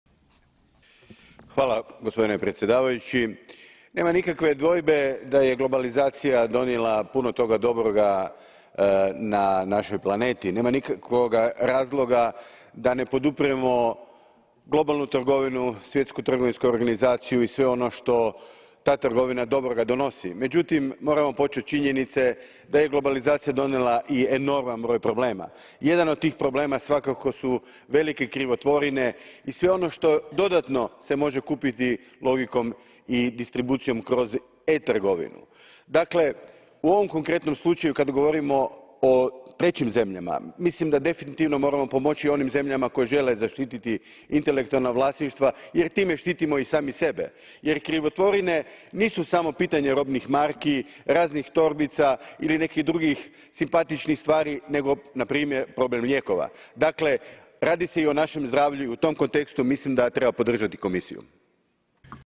Ivan Jakovčić 08 Jun 2015 plenary speech on Intellectual property rights
Ivan Jakovčić 08 Jun 2015 plenary speech on Intellectual property rights in third countries